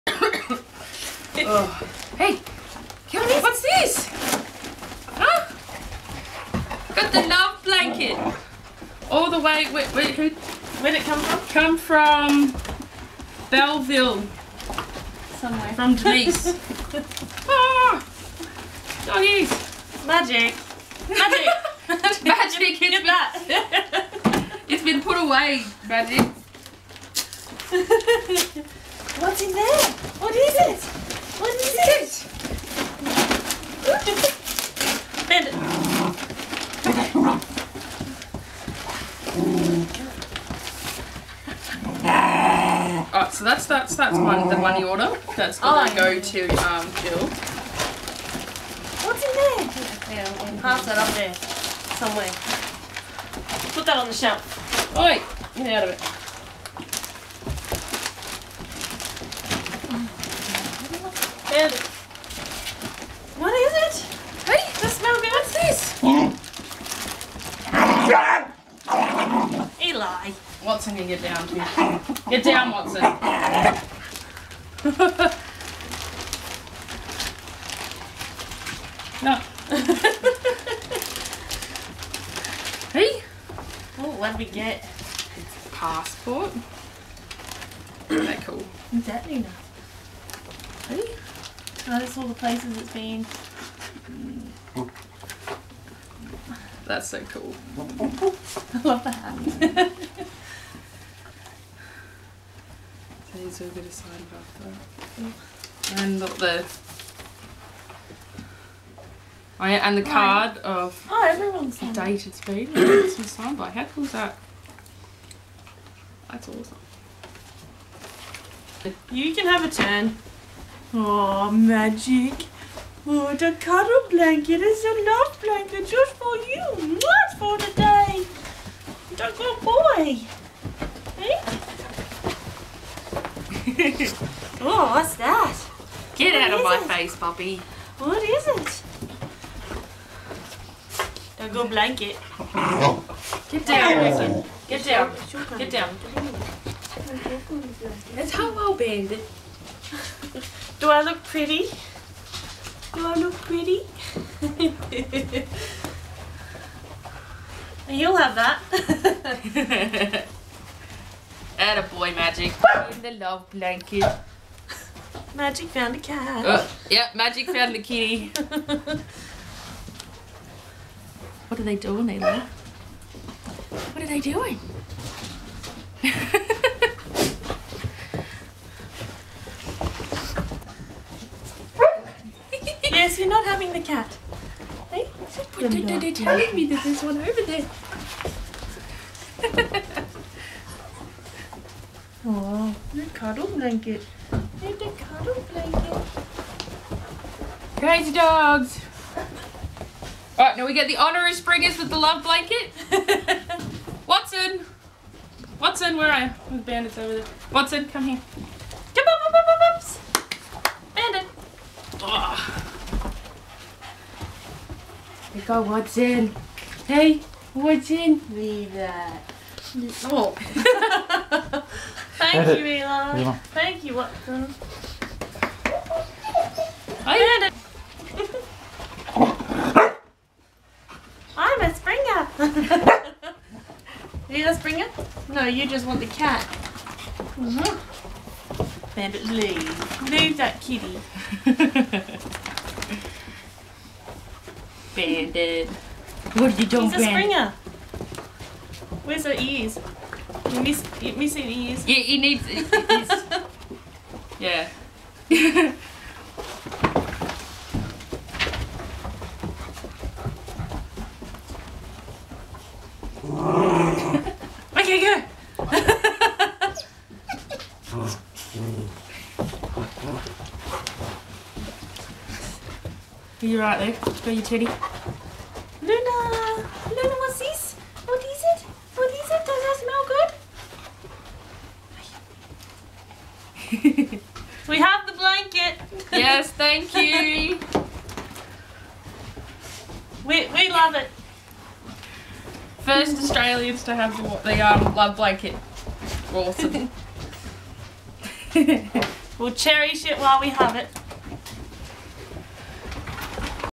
LOVE BLANKETS FIRST STOP IN AUSTRALIA
Hello All, Jess and I filmed the opening and first cuddles with the Love Blanket. yes we are odd on video (well i am) and the pooches were very excited (my 3 not used to cats and very curious lol) Welcome to Australia hehehe :-) Enjoy our sillyness lol